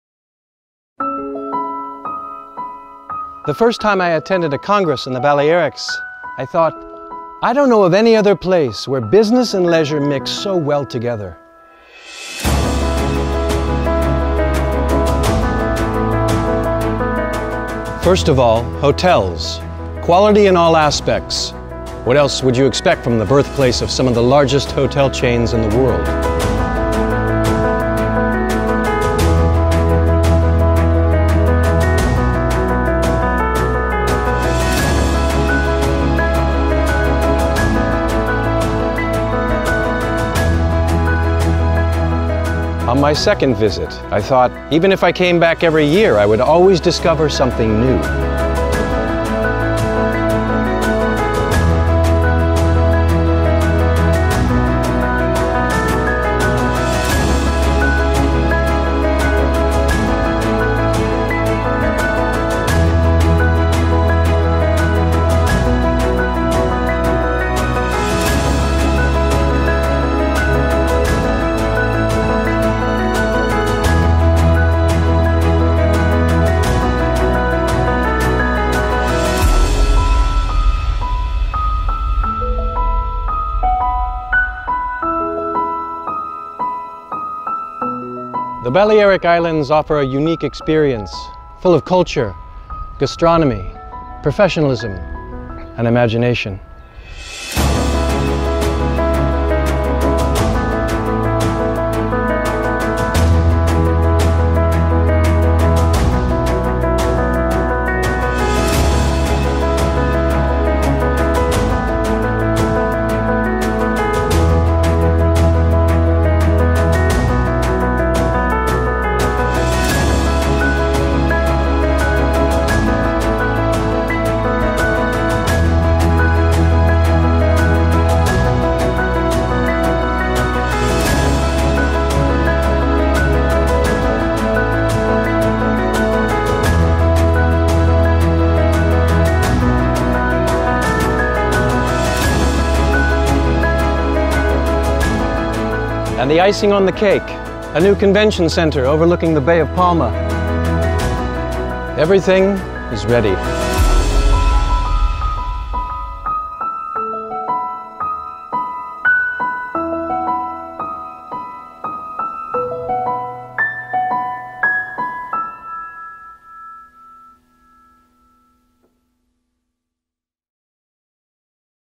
BALEARIC ISLANDS | A paradise also in MICE
Mallorca, Menorca, Ibiza, and Formentera are an ideal place to celebrate Meetings, Incentives, Congress and Events.